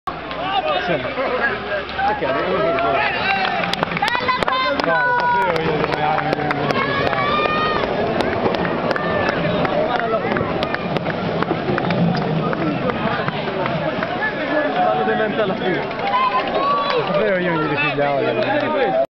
Parate Carretti Allo Sterzo
Parata di tutti i carretti allo sterzo.